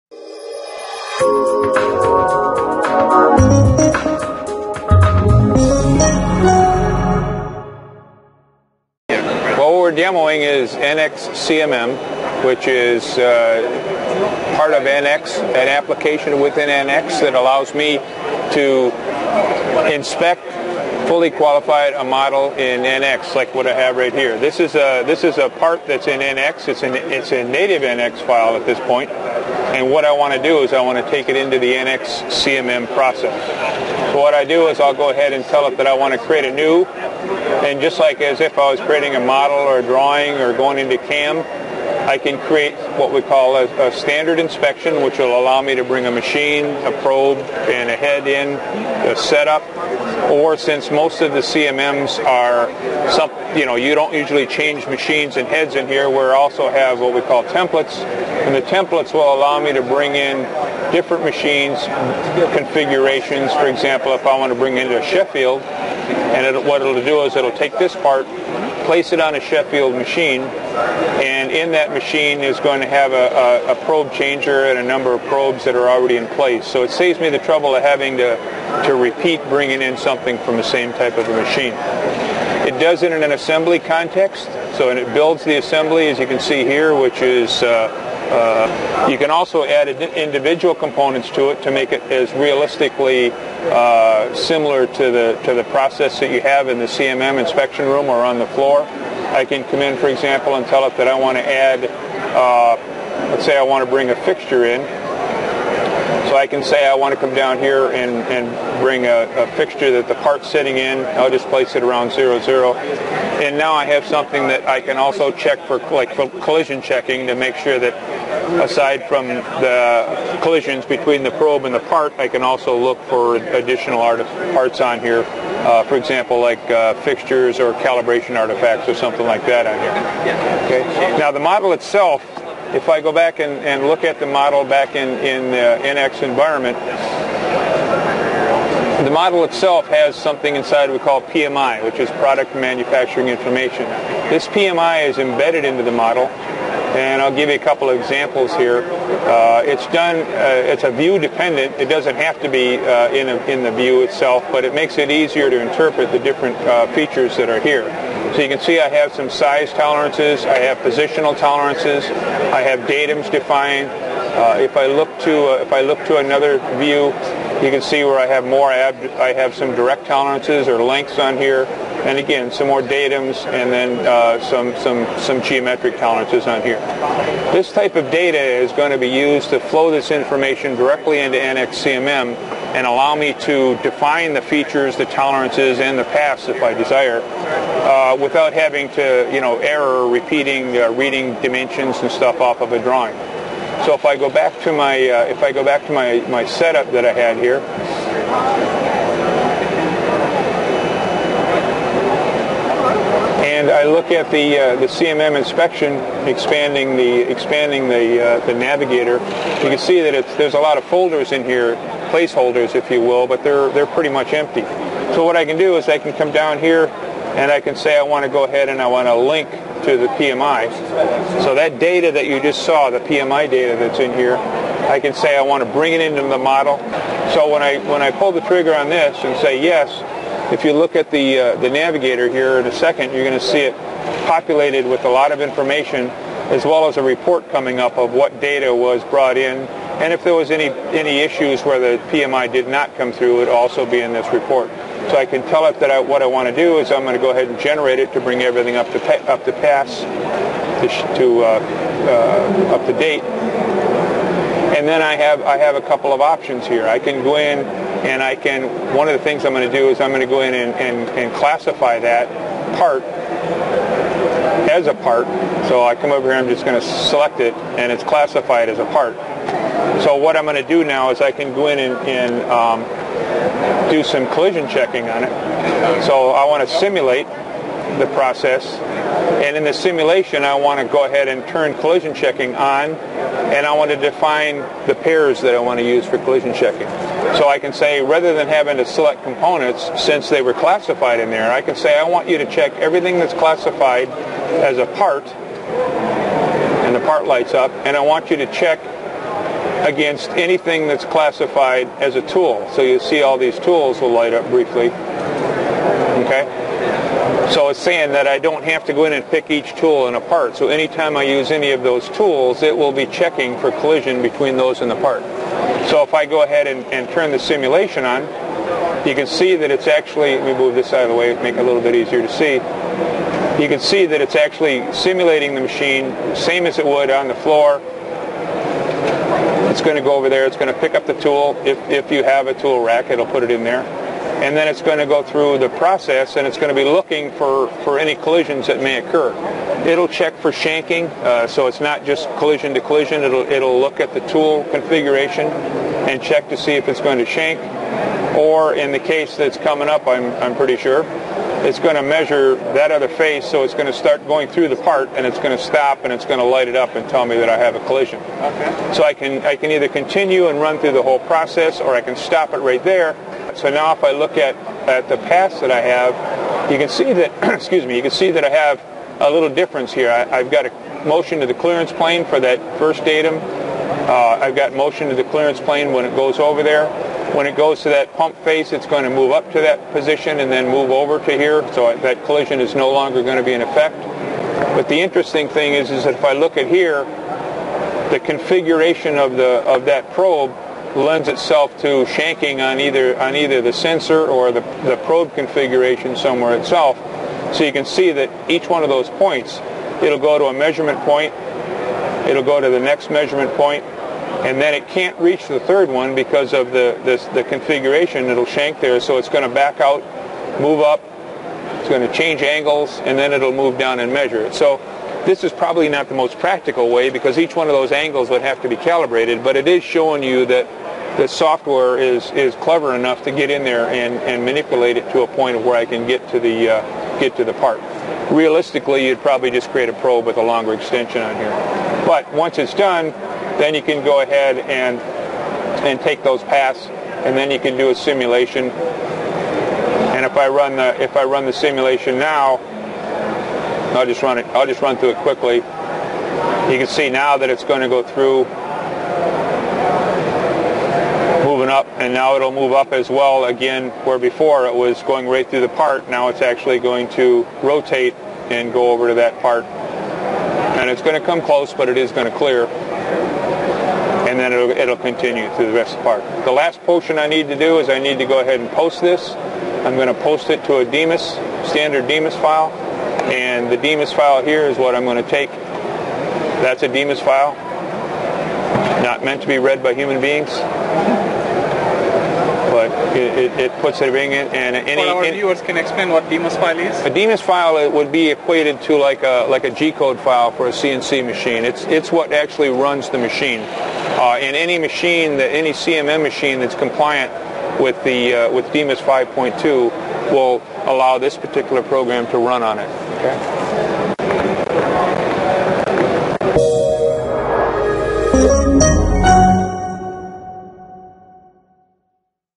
NX CMM Demonstration at IMTS 2012
Watch Rick Seymour demonstrating NX CMM programming at IMTS 2012